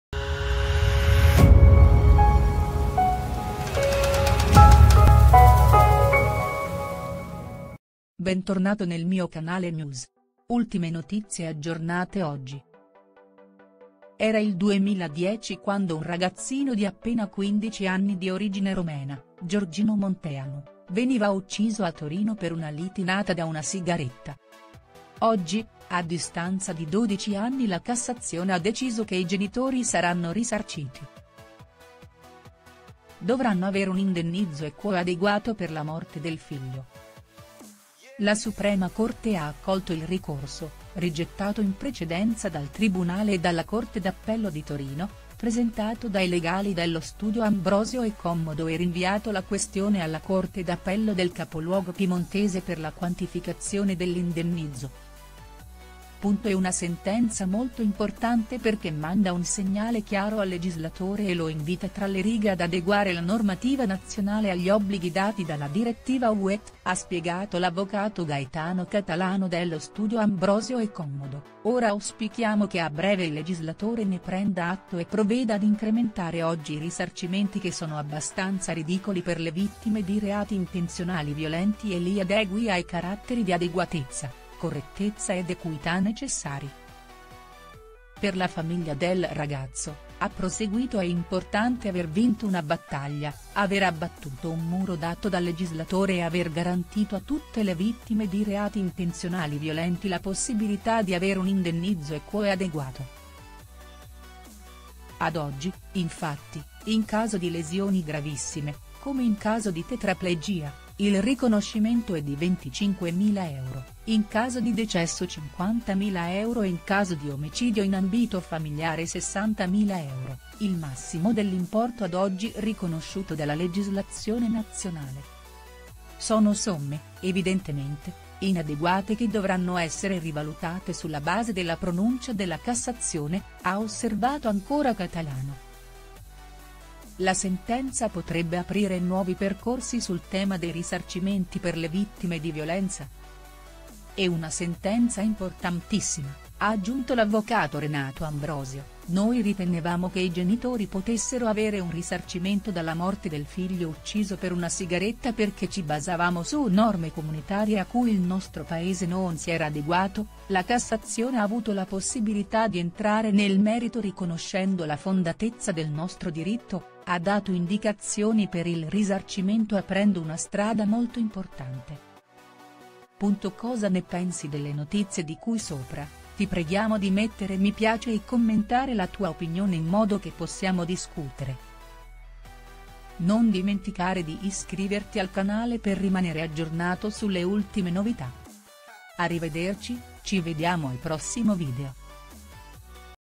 Ragazzino ucciso a coltellate per una sigaretta, i genitori avranno risarcimento dopo 12 anni
Era il 2010 quando un ragazzino di appena 15 anni di origine romena, Giorgino Monteanu, veniva ucciso a Torino per una lite nata da una sigaretta. Oggi, a distanza di 12 anni la Cassazione ha deciso che i genitori saranno risarciti.  Dovranno avere un indennizzo "equo e adeguato" per la morte del figlio.  La Suprema Corte ha accolto il ricorso, rigettato in precedenza dal Tribunale e dalla Corte d'Appello di Torino, presentato dai legali dello studio Ambrosio e Commodo e rinviato la questione alla Corte d'Appello del capoluogo piemontese per la quantificazione dell'indennizzo.